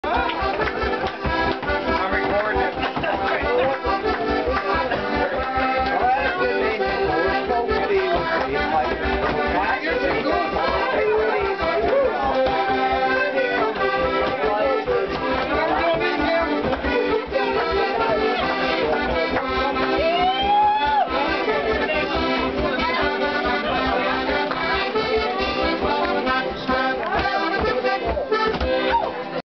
Newfoundland Shed Party
November 22 2007 Paradise Newfoundland, shed party at Rufus Hamiltons house.